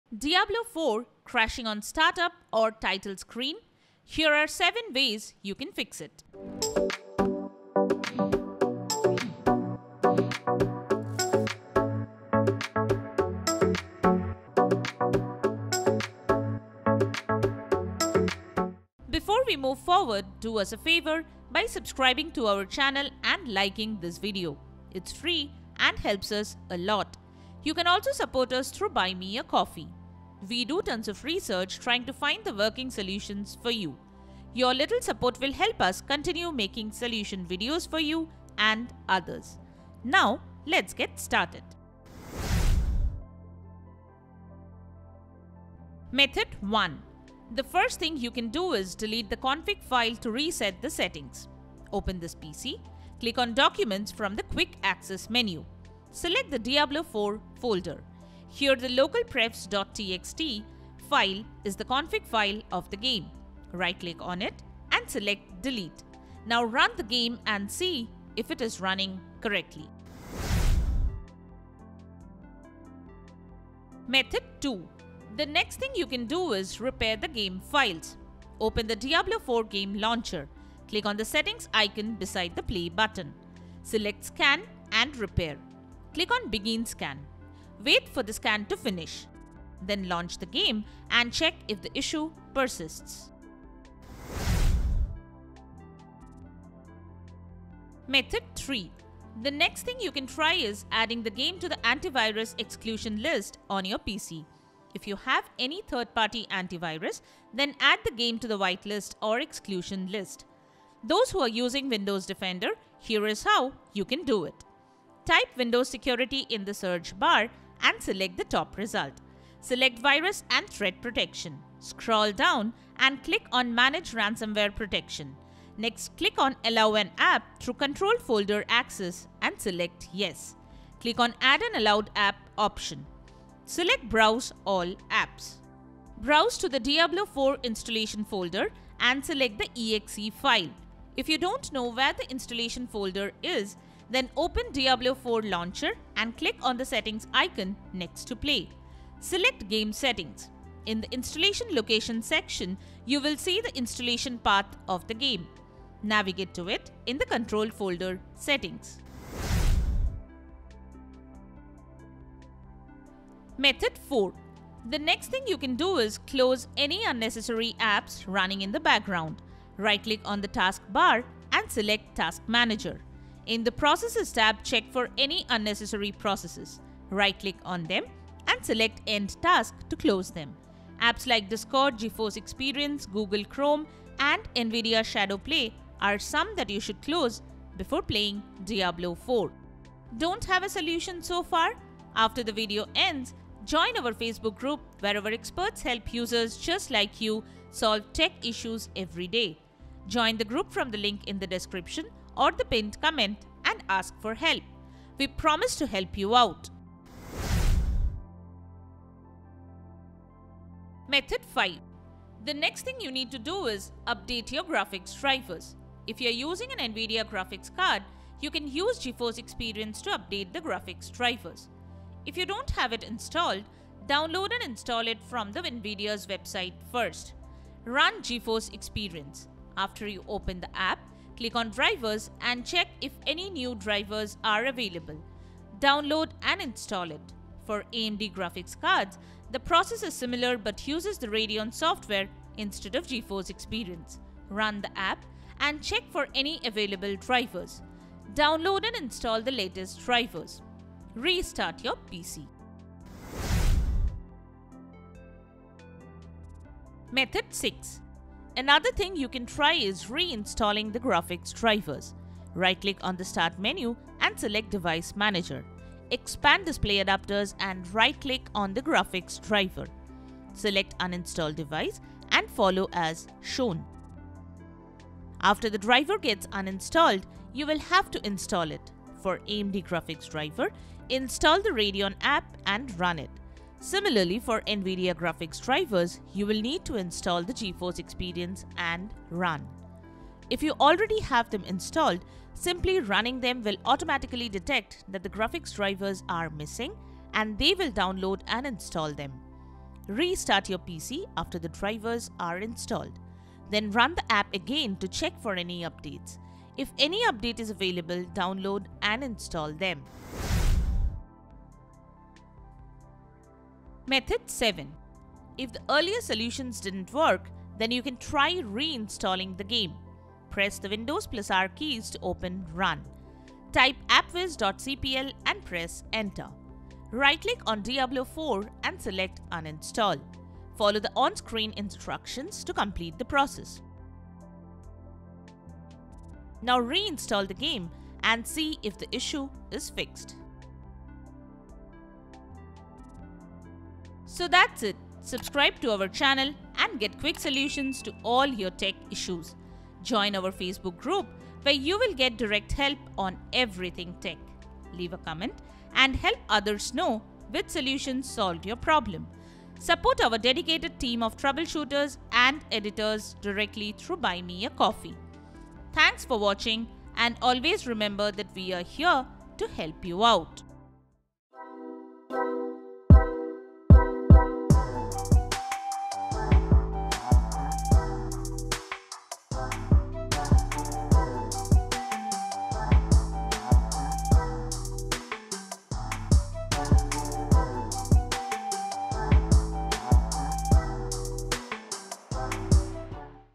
Diablo 4 Keeps CRASHING ON STARTUP or Title Screen || Diablo 4 WON'T START [7 TIPS]
Diablo 4 keeps crashing on startup or title screen?
Here we have shown 7 working methods to fix Diablo 4 Won't Start.

Timestamps:
00:00 Intro
00:44 Delete Config Files
01:12 Repair Game Files
01:36 Add Game to Exclusion List
02:41 Close Unnecessary Apps
03:29 Update Graphics Driver
04:15 Re-install Graphics Driver
05:12 Re-install Game